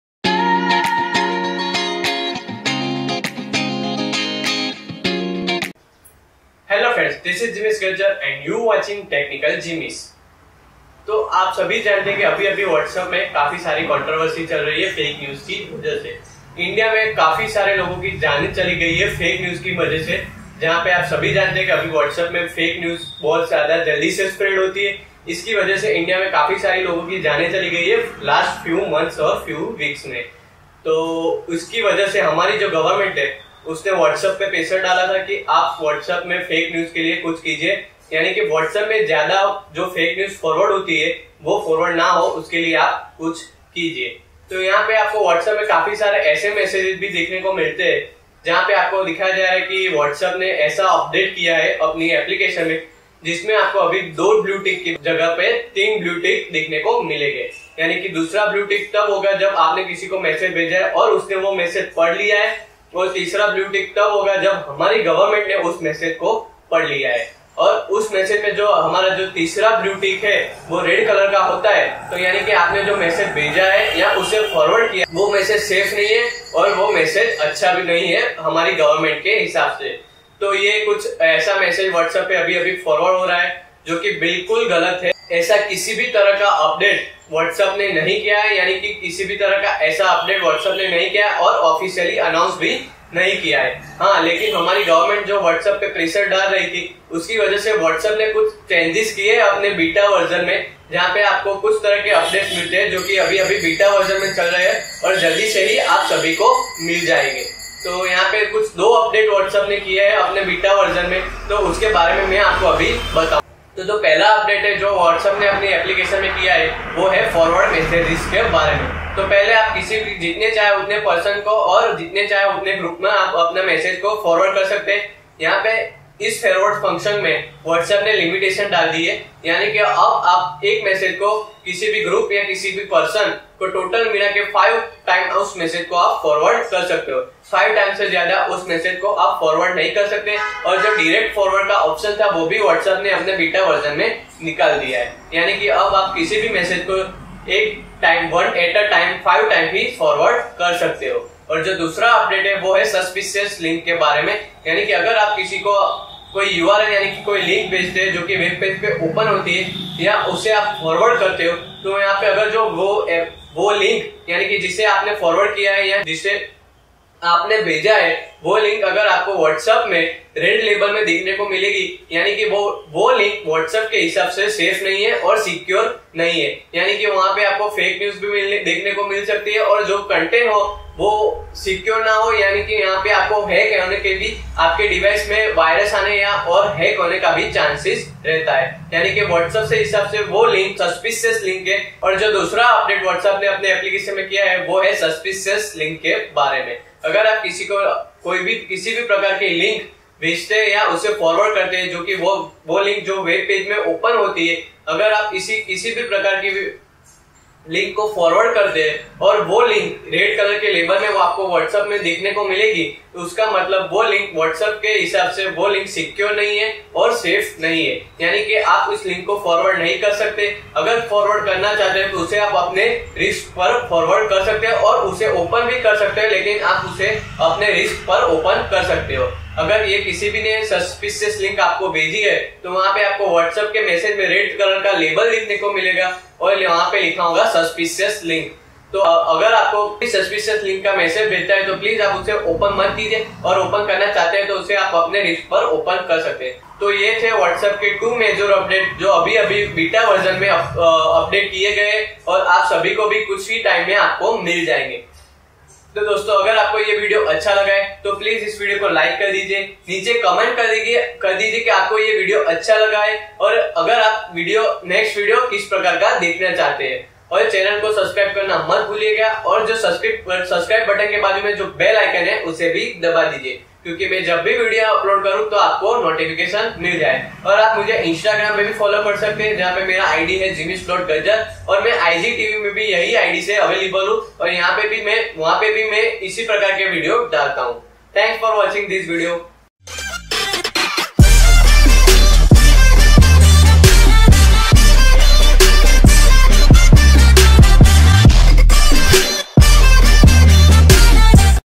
Whatsapp Message Read By Govt | Whatsapp Three Bluetick | Fake Message New Update.
In this video i am talking about whatsapp new message forwarding & suspicious link update and also talking about fake news spread on whatsapp three blue tick & Government read   our message.